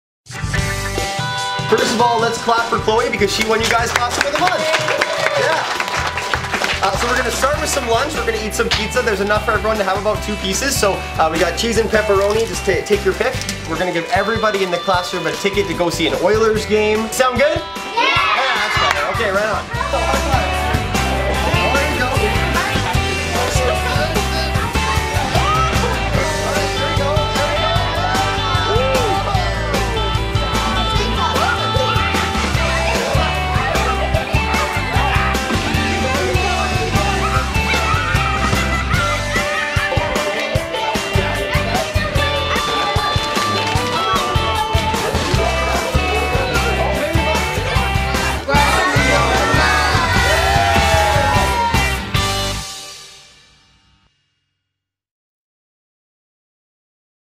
Classroom of the Month - September 2016
We headed to Big Rock School and hung out with Clohey and her Grade 1 class after she won them a party with Classroom of the Month!